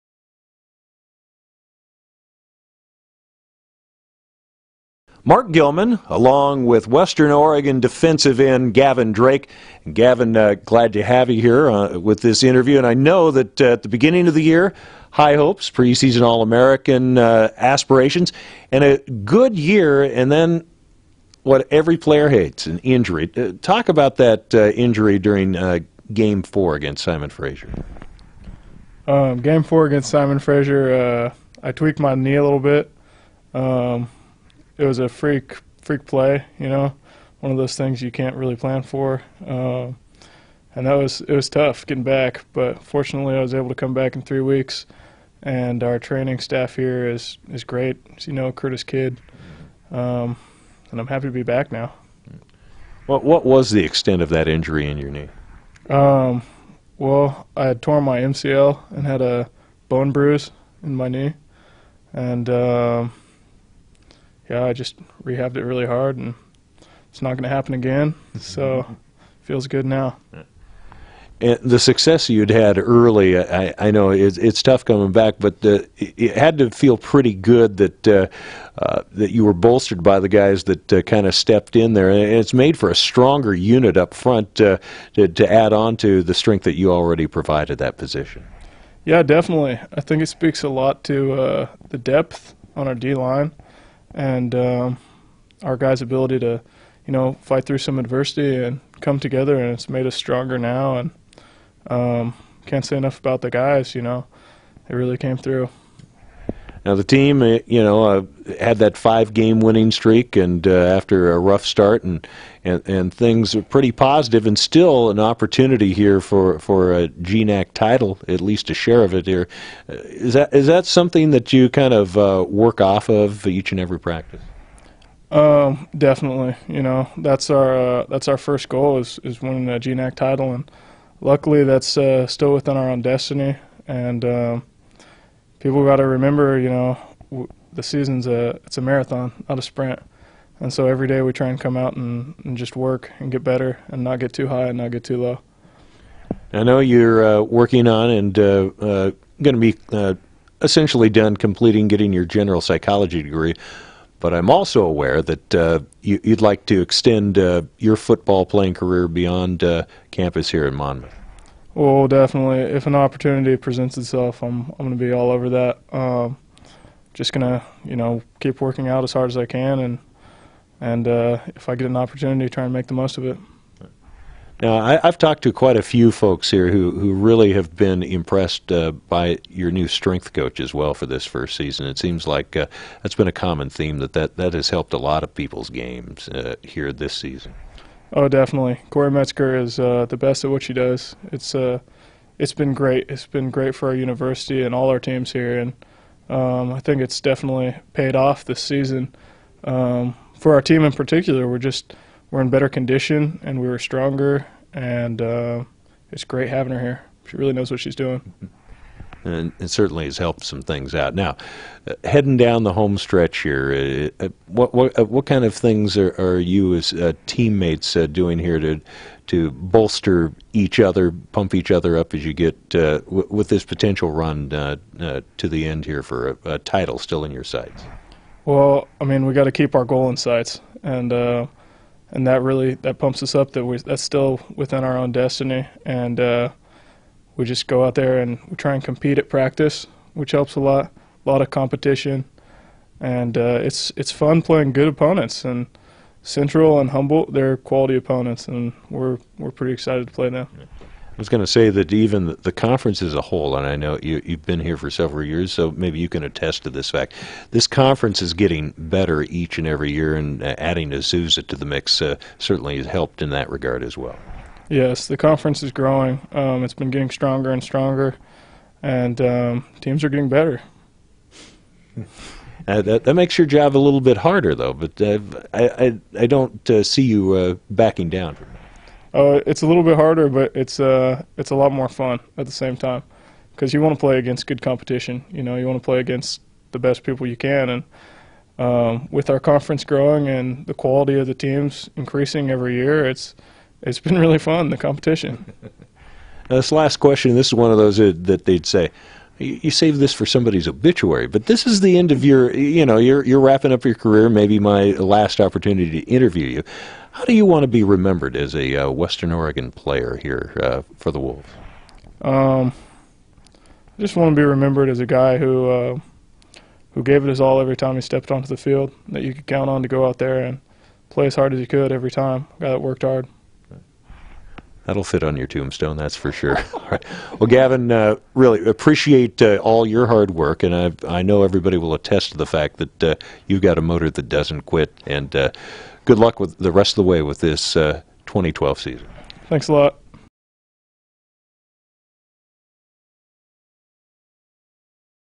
Western Oregon Defensive End Gavin Drake
Full Access Sports' Mark Gilman chats with Western Oregon defensive end and pre-season All-American Gavin Drake about his career and the Wolves' roller coaster season.